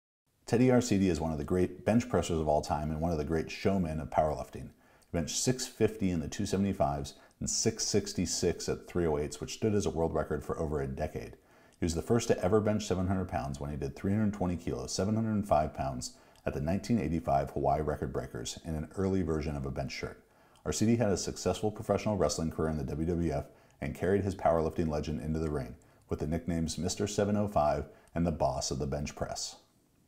Professional Wrestler Benching 700 Pounds!
Ted Arcidi is one of the greatest bench pressers of all-time, doing 650@275 and 666@308 which stood as World Record for over a decade. He was also the first ever to bench 700 when he did 320kg/705# at the 1985 Hawaii Record Breakers in an early bench shirt. He then went on to a career as a professional wrestler in the WWF.